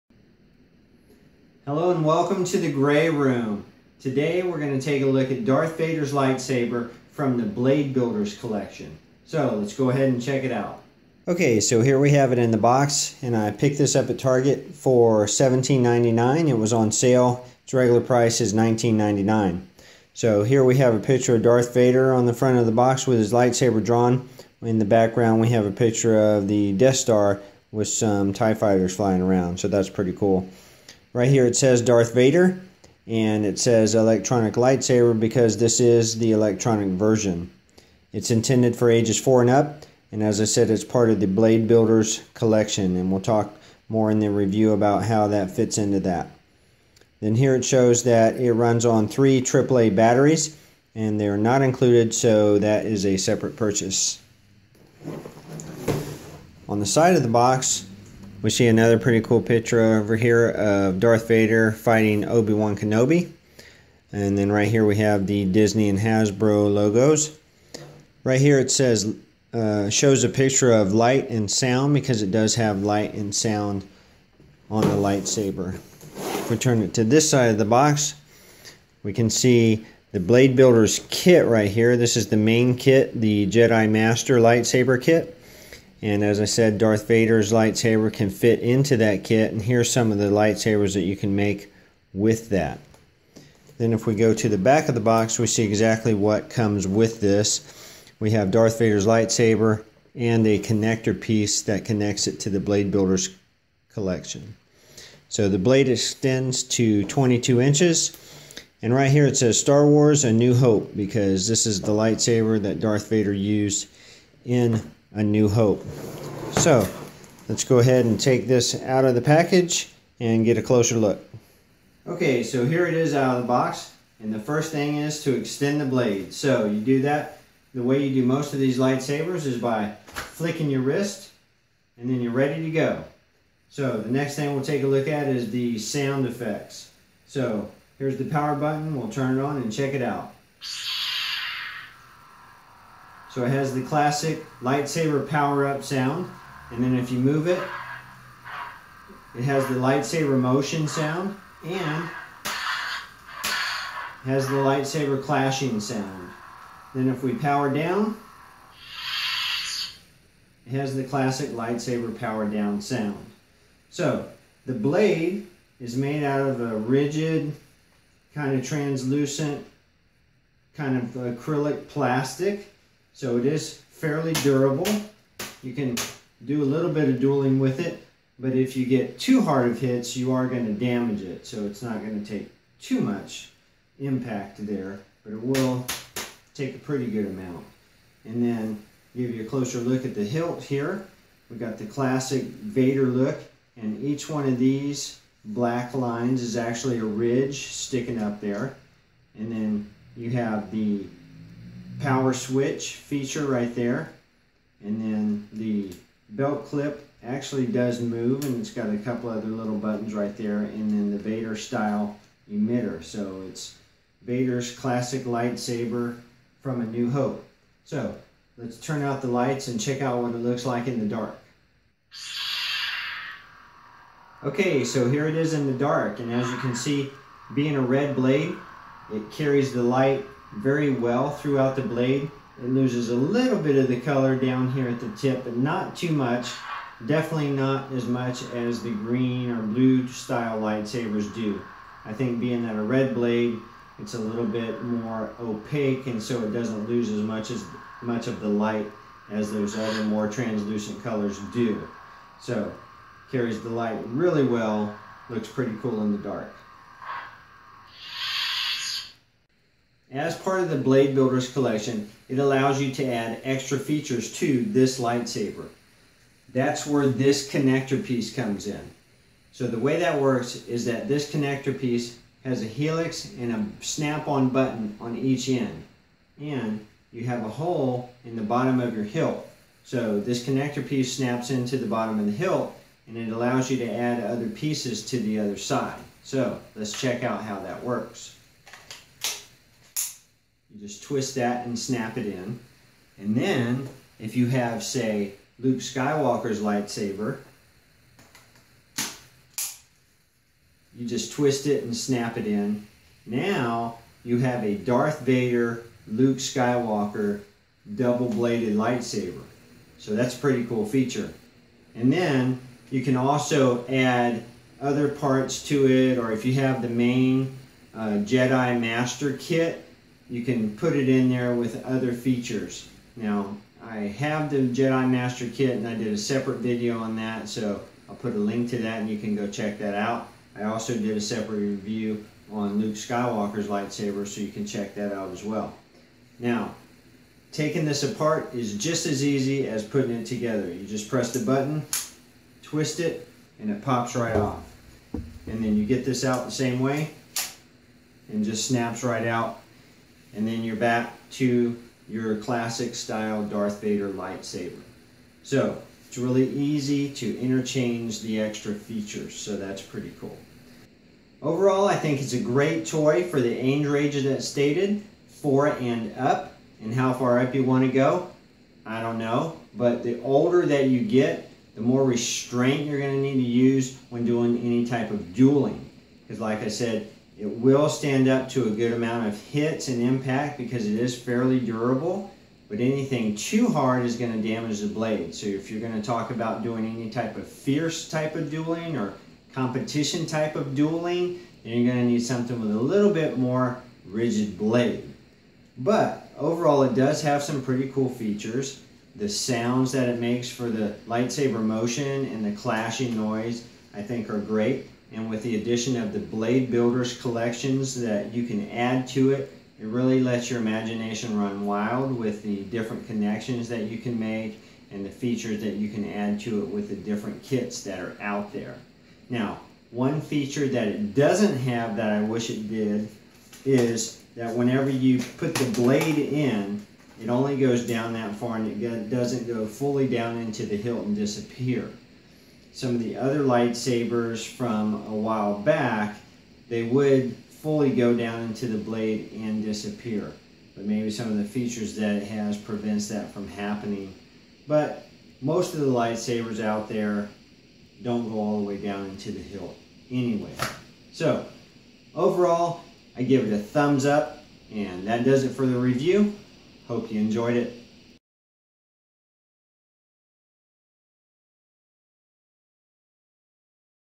Star Wars BladeBuilders Darth Vader's Lightsaber Review
This is a review of Darth Vader's Lightsaber from Star Wars a New Hope. It is from Hasbro and is part of the BladeBuilders Lightsaber collection. This review has not been approved or authorized by any entity or person(s) associated with the creation/production of Star Wars or Star Wars items.

The Gray Room:
The place for Comics, Collectibles and Life in General